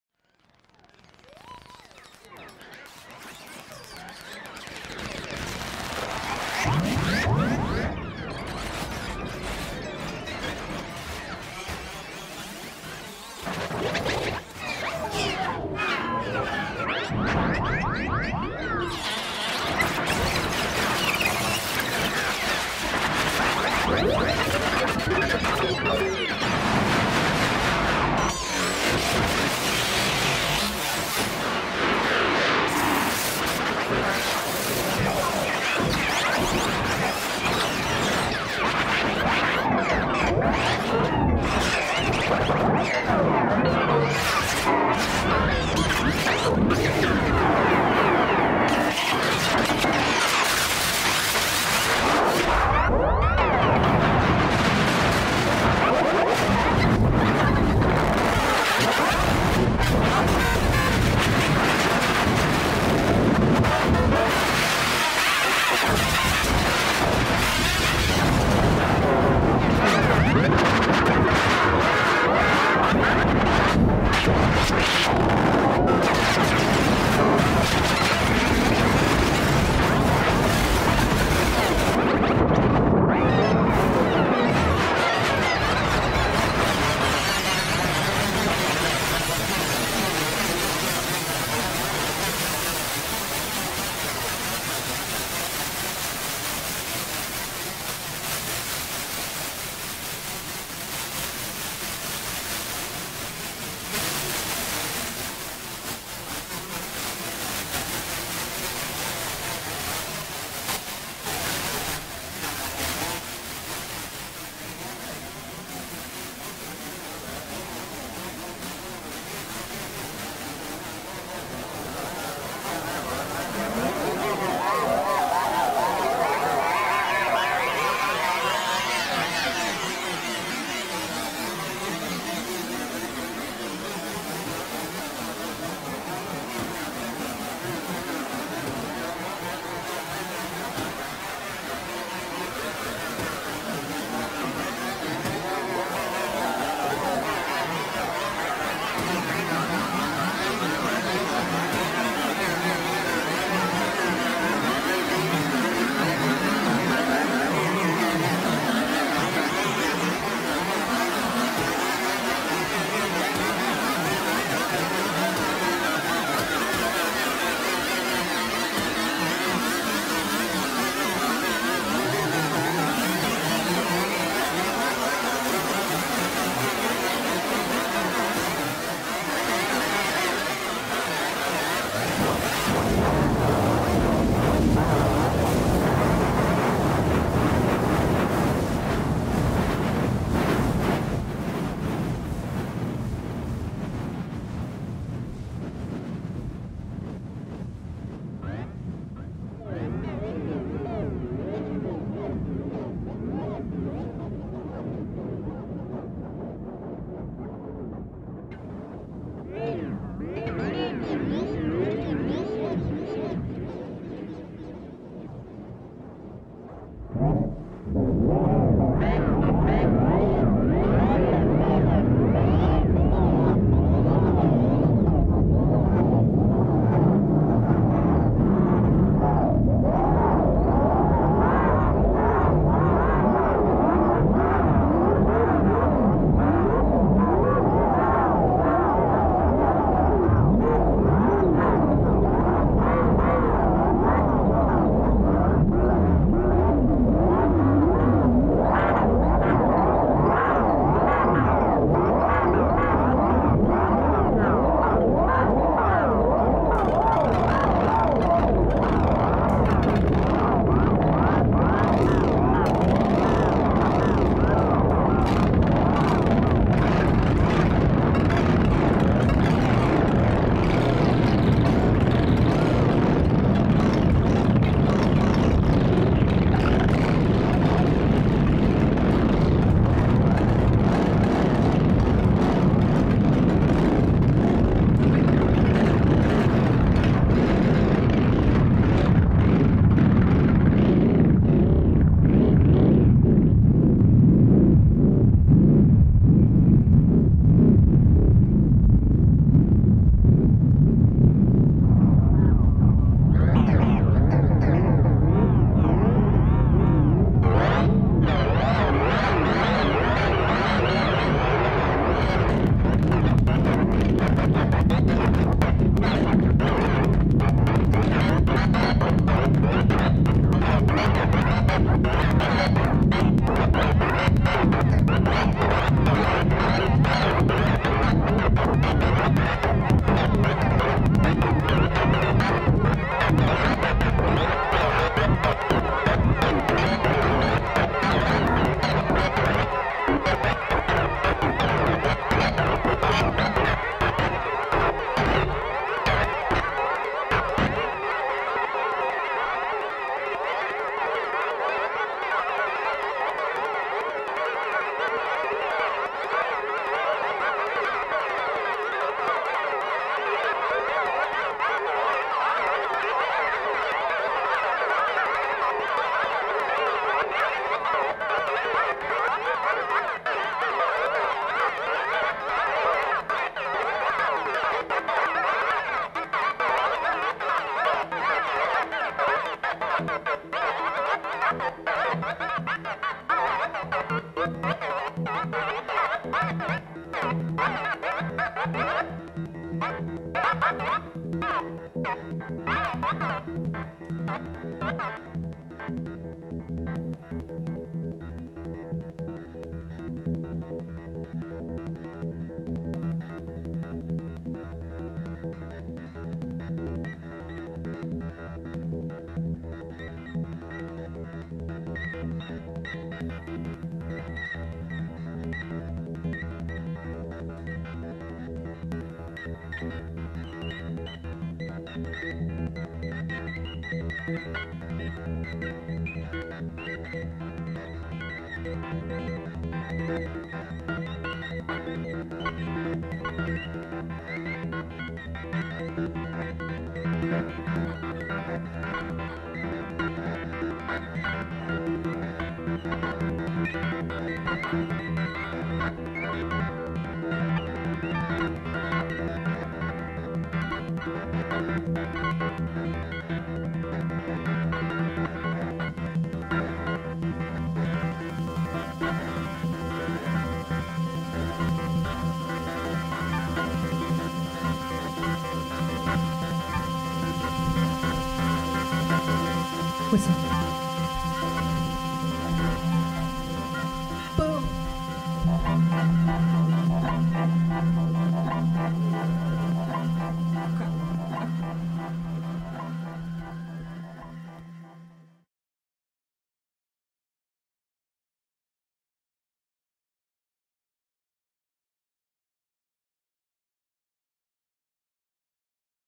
Endangered Guitar at 3DMIN Concert Berlin 2016
Solo contribution at the 3DMIN concert evening as part of "Musical Instruments in the 21st Century" symposium.

Excerpt of the entire concert with Alberto de Campo, Alexander Peterhänsel, Amelie Hinrichsen, Atau Tanaka, Dafna Naphtali, Dominik Hildebrand Marques Lopes, Hannes Hoelzl, Hans Tammen, hans w. koch, Yan Jun, Katharina Hauke, and Till Bovermann